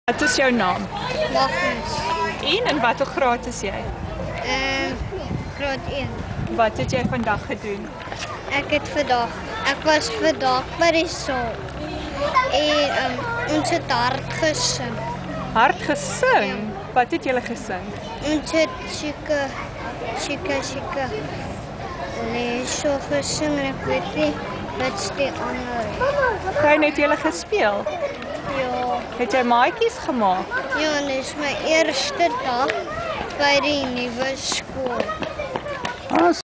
My eerste dag by die Laerskool George-Suid3.MPG
Die eerste skooldag by die "groot skool" kan vreesaanjaend wees vir die nuwe graad eentjies. Eden Express het 'n paar van hulle gevra hoe dit was.